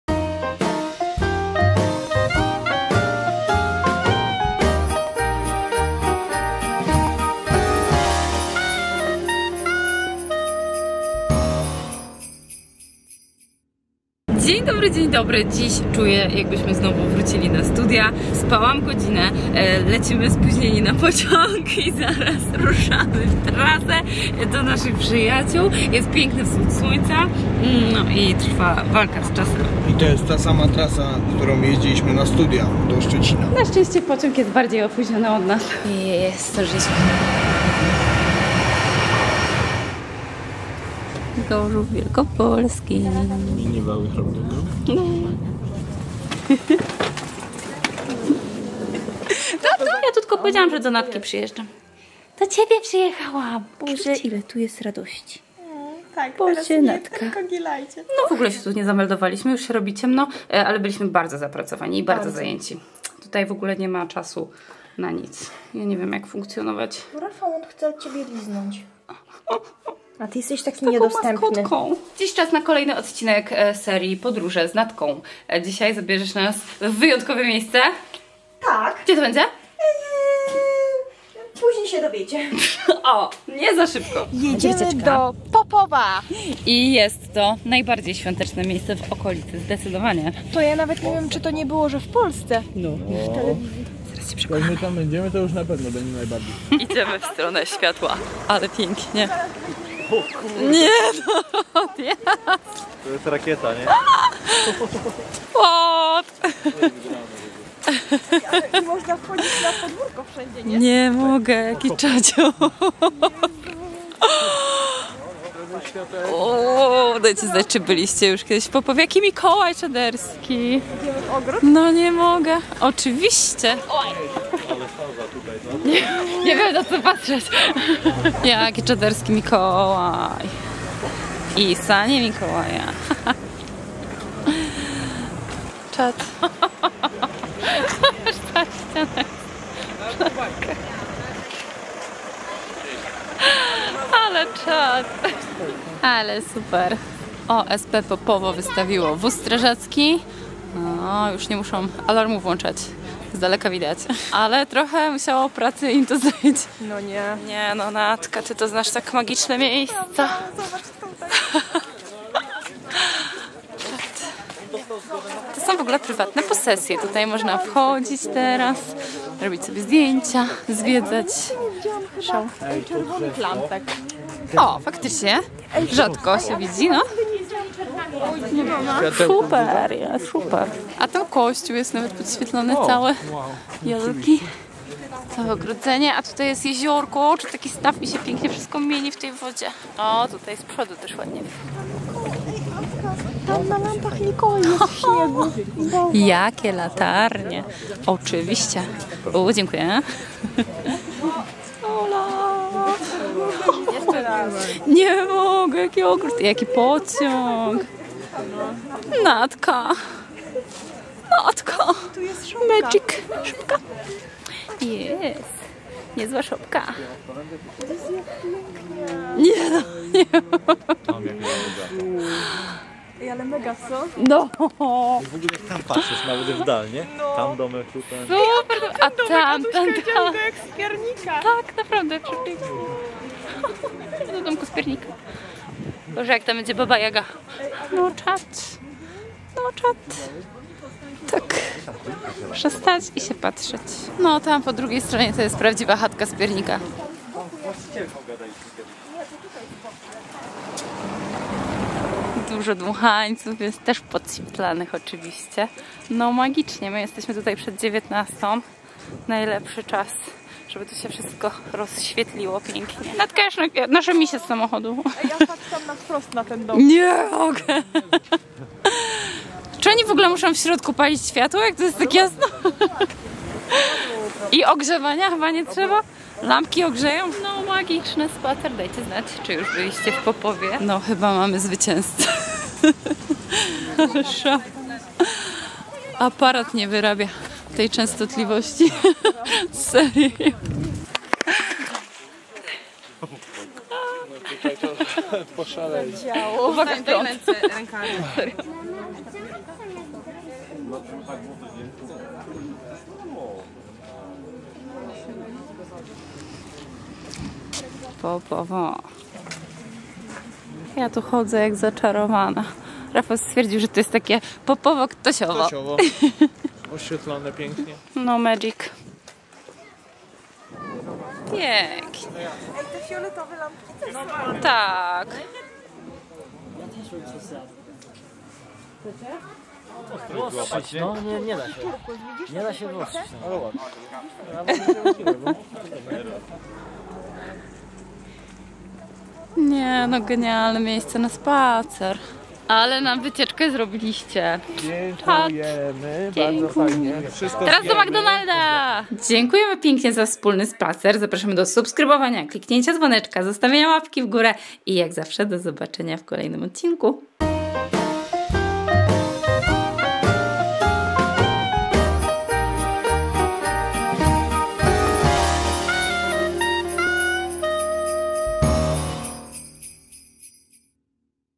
POPOWO najbardziej rozświetlona wieś w Polsce
Odwiedziliśmy Popowo - najbardziej rozświetloną wieś w Polsce! Nie zapomnij o kciuku w górę!